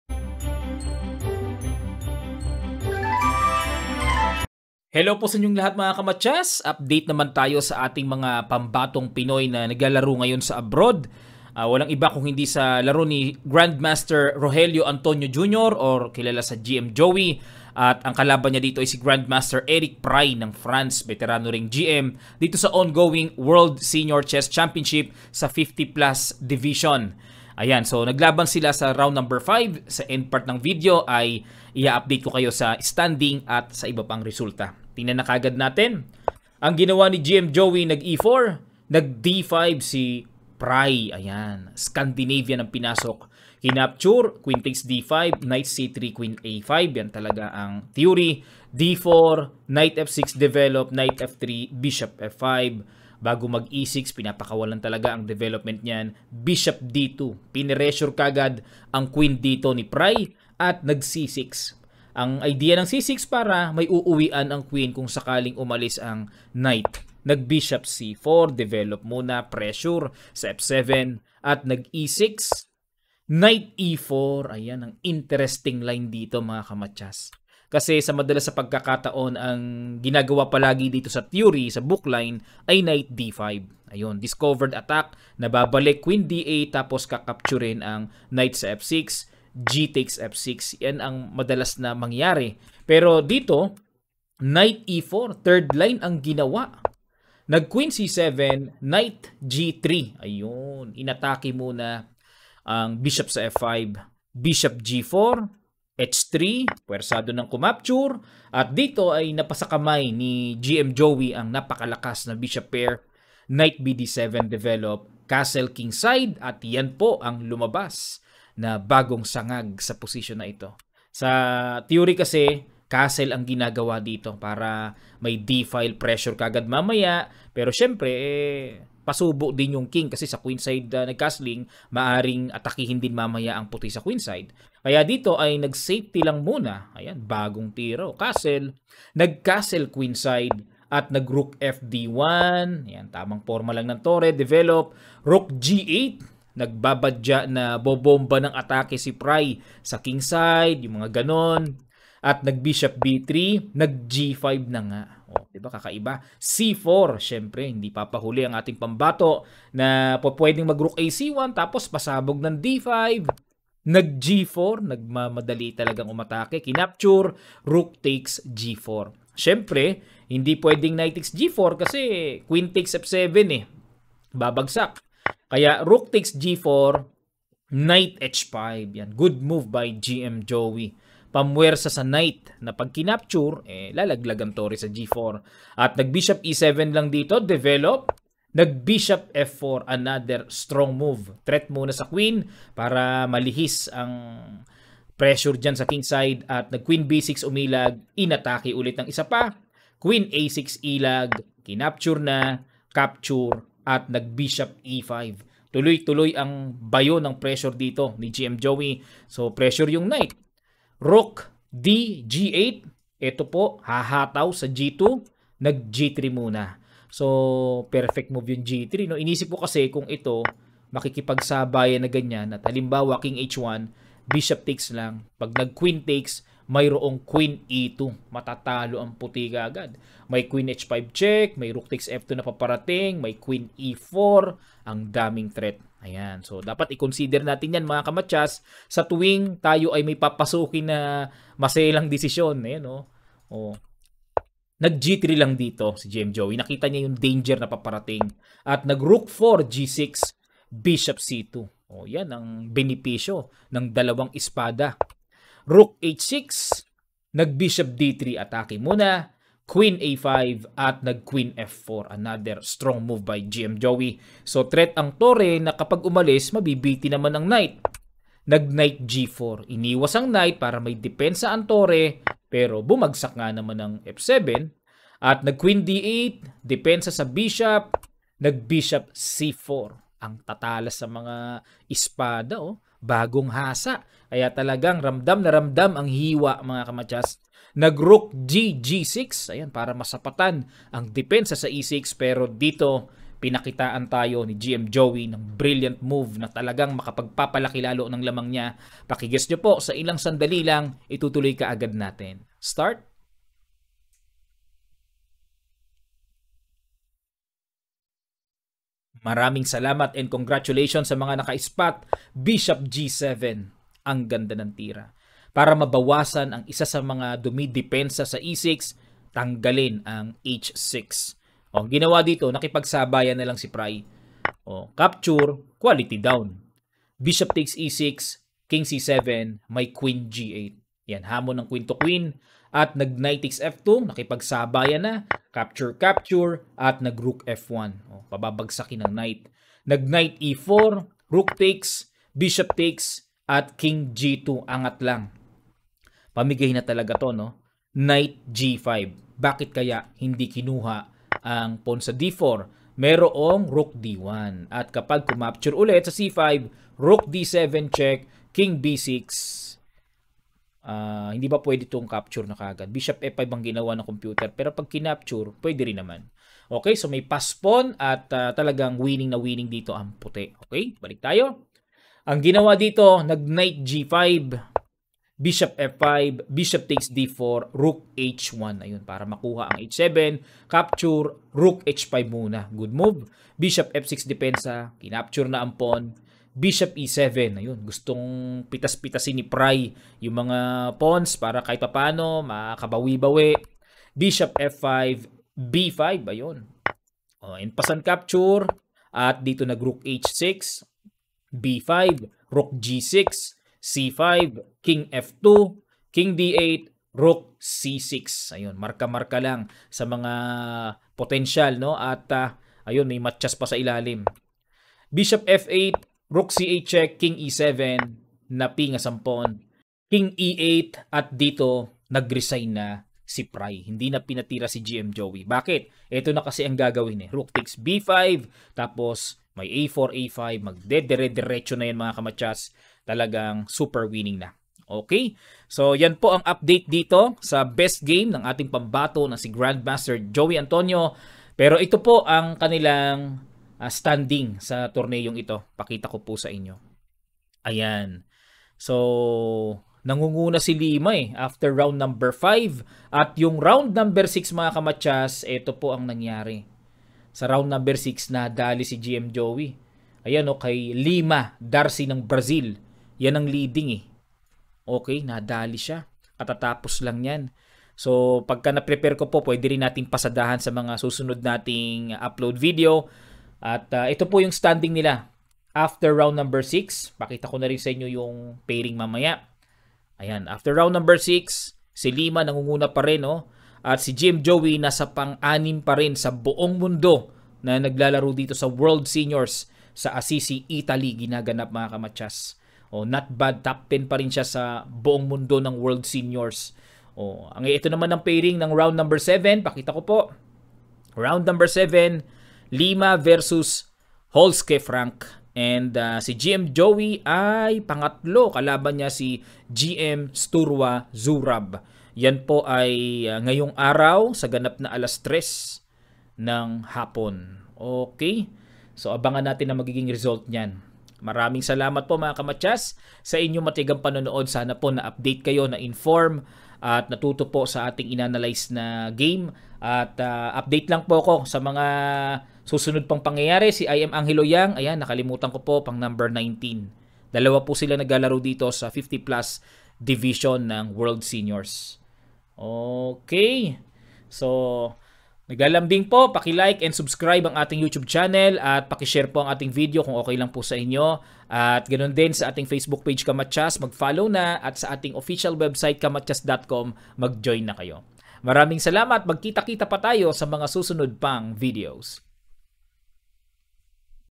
UPDATE KAY FILIPINO GM Rogelio "Joey Antonio sa World Seniors Chess Championship 2022 50+ Division.
Pa support mga Kamatyas! Please mhwaa mhwaa!

Kung nakakaluwag po sa buhay, pwede rin mag share ng Blessings. God Bless po!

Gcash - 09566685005